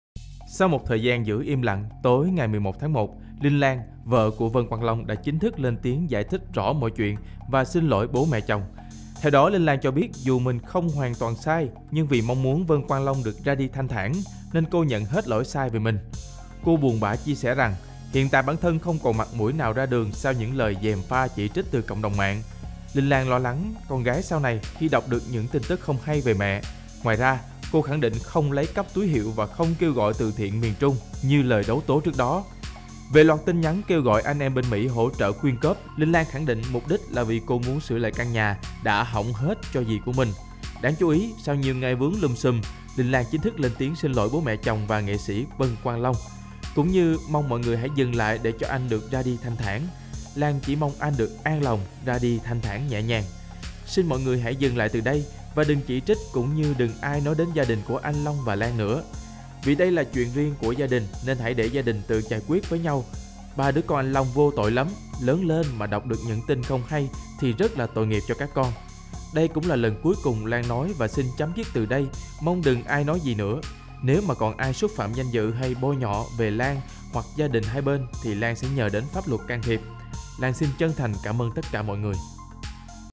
Muốn chồng ra đi thanh thản, Vợ 2 Vân Quang Long nhận hết lỗi về mình
Sau nhiều ngày giữ im lặng, Linh Lan - vợ Vân Quang Long chính thức xin lỗi chồng và bố mẹ chồng, giải thích ngọn ngành loạt thông tin bị đấu tố trên MXH